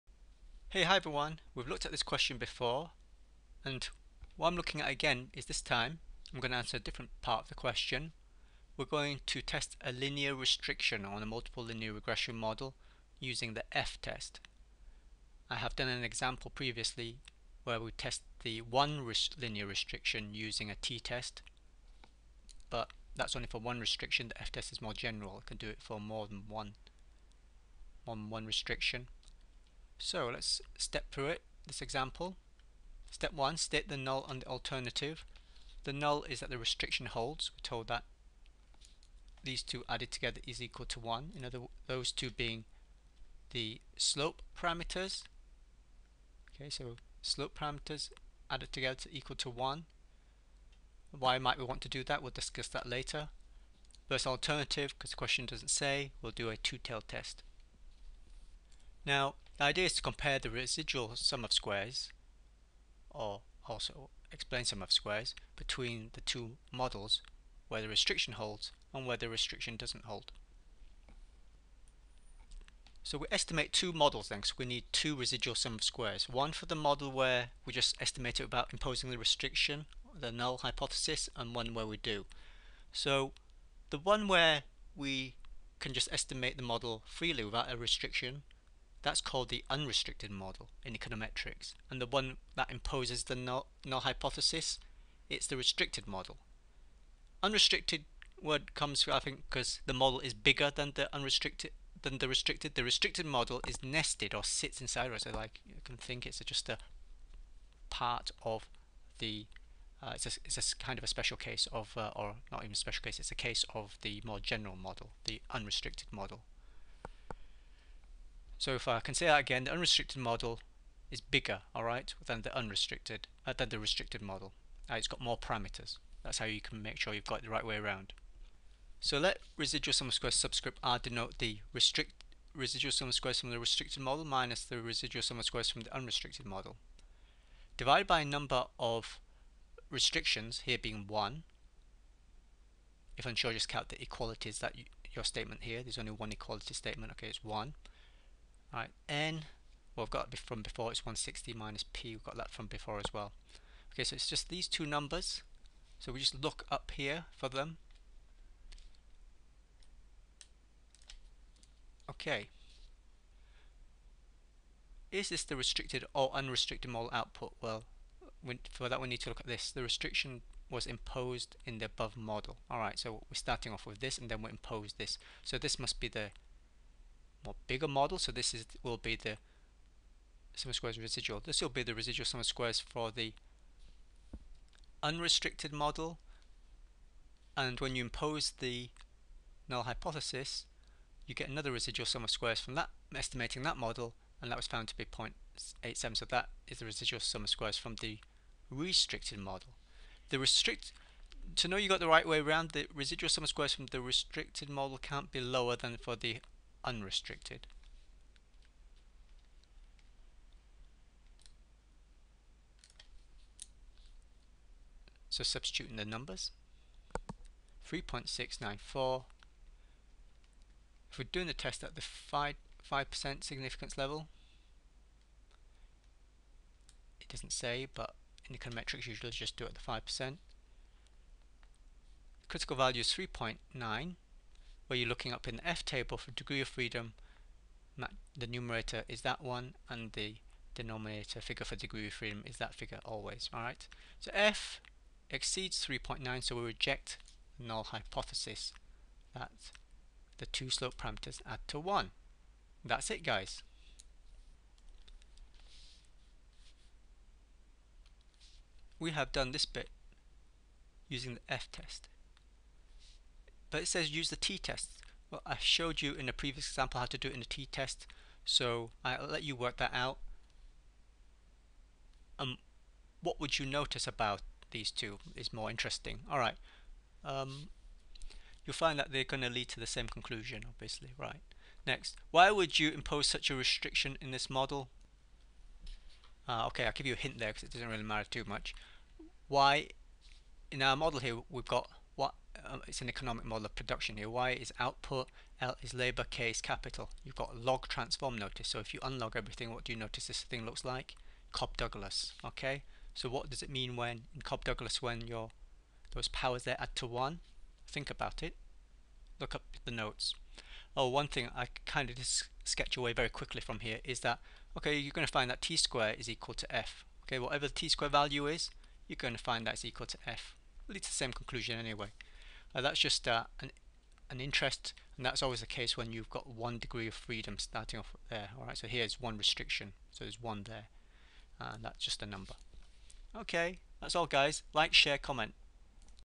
#13 F-test of linear restriction in regression example
F-test of linear restriction on the coefficients, using formulation based on residual sum of squares